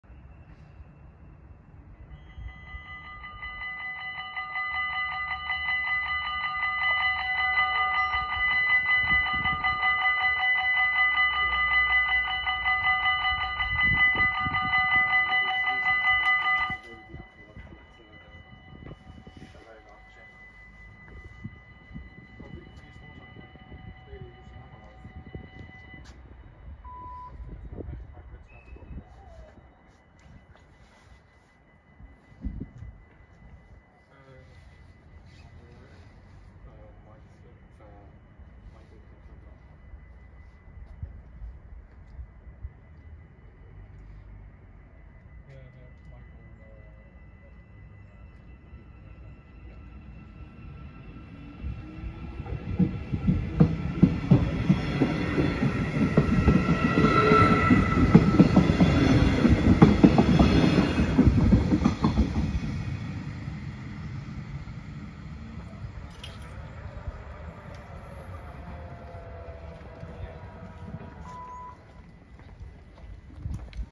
Spoorwegovergang Hardenberg Nacht Editie // Dutch railroad crossing Night Edition
Locatie: Station Hardenberg
Traject: Spoor 1
Soort: AOB
Bellen: 2
Bomen: 2
Passeren:
- GTW-E als RS20 Emmen → Zwolle